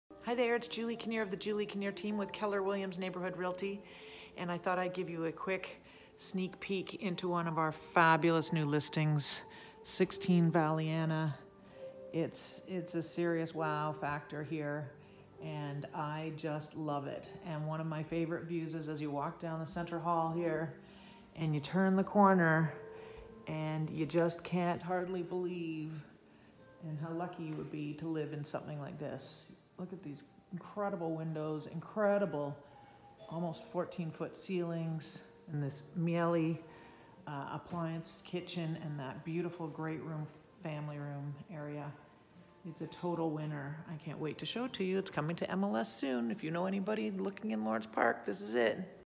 Julie and The Fabulous New Listing
In this week's video Julie shows a little sneak peek of the fabulous upcoming listing.